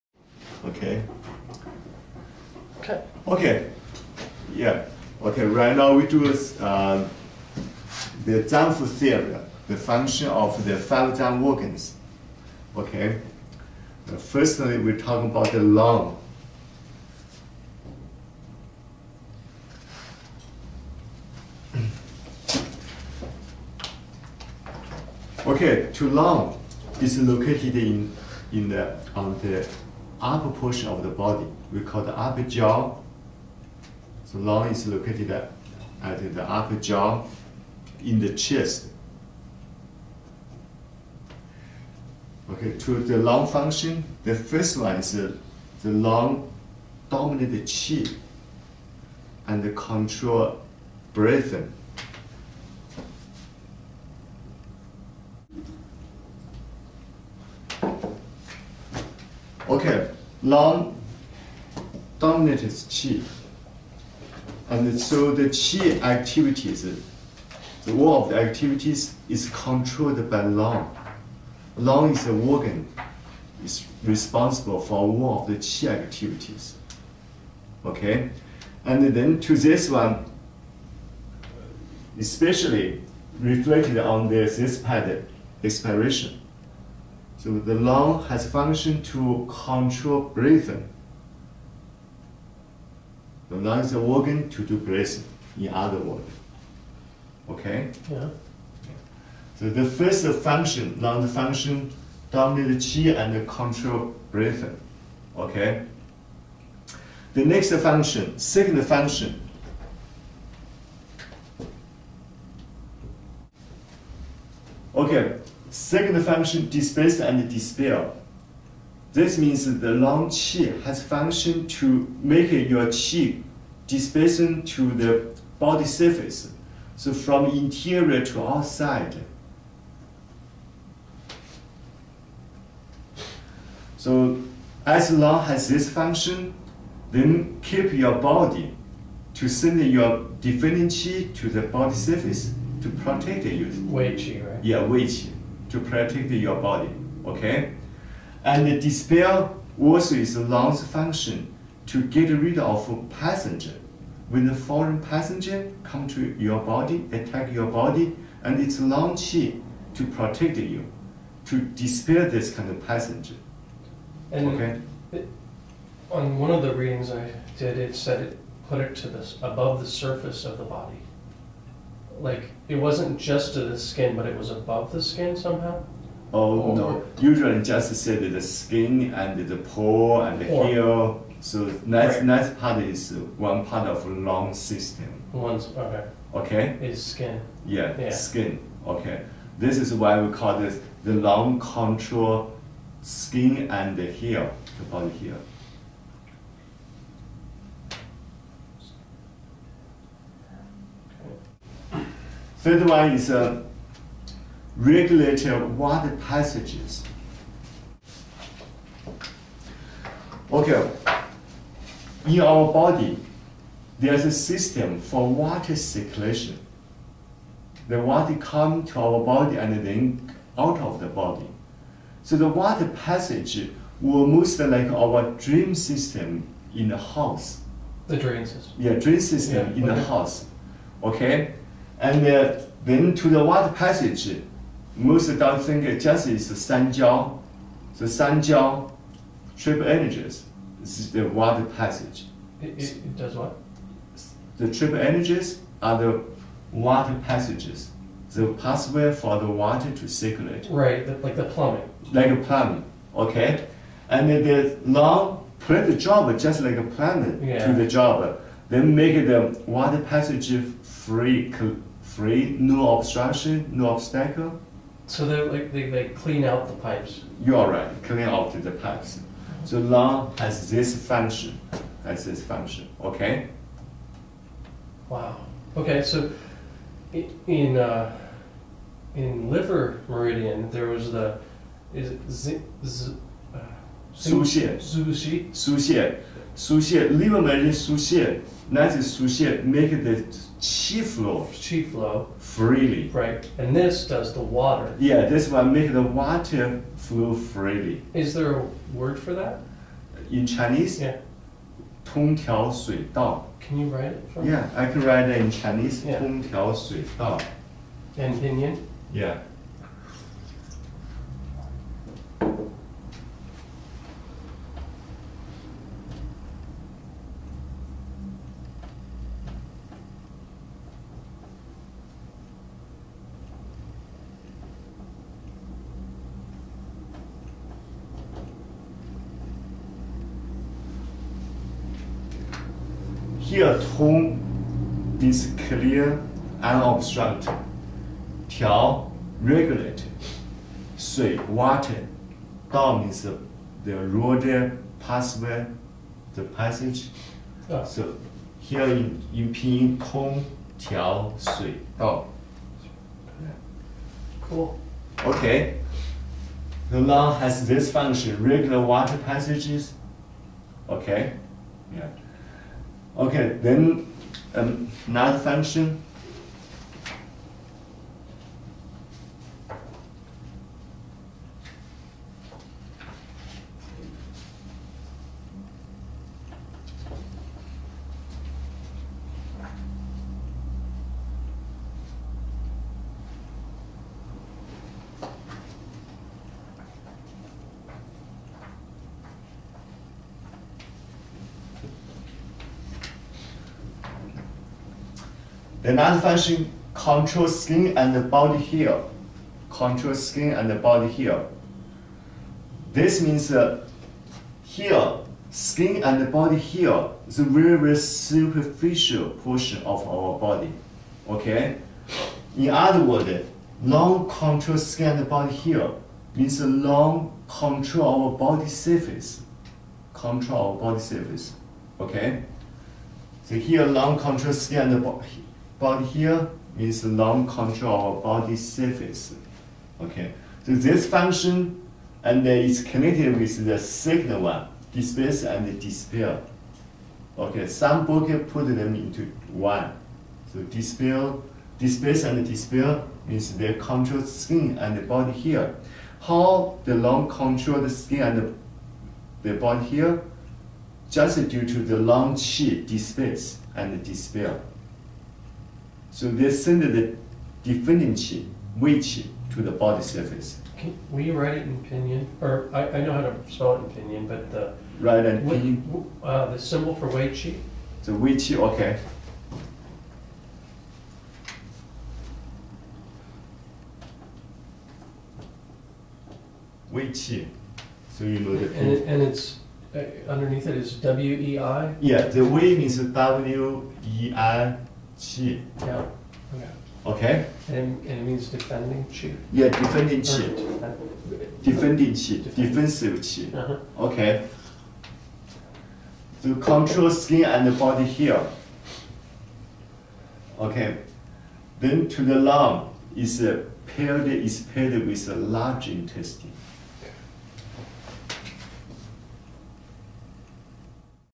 Zang Fu Theory with subtitles (part 1)
Zang Fu Theory Part 1  Dr. Ganglin Yin of Oshio College of Acupuncture and Herbology